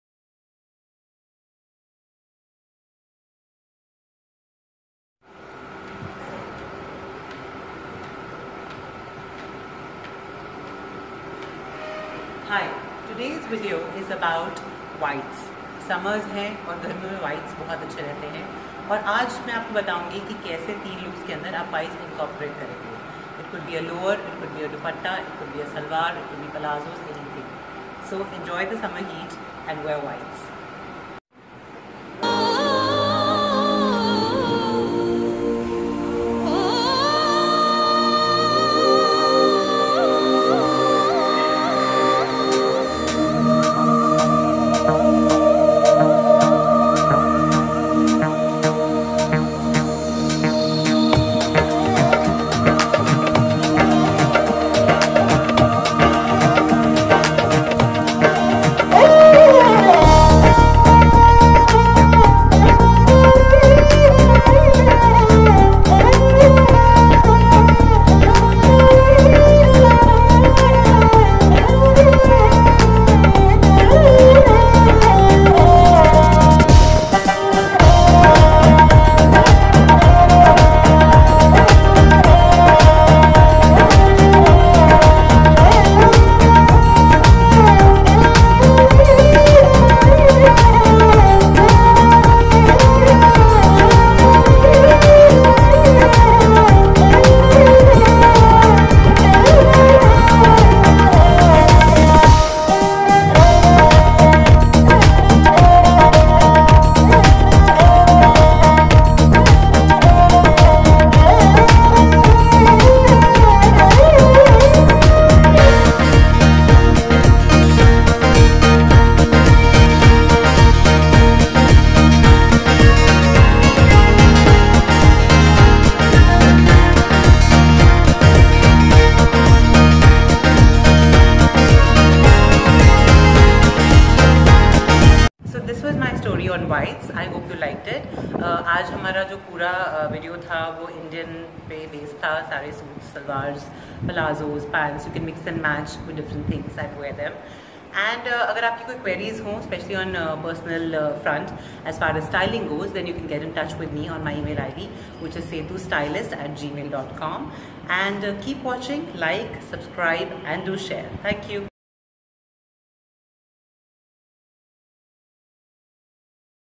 WHITES- Indian Avatar
Three looks - incorporating whites into your Indian wardrobe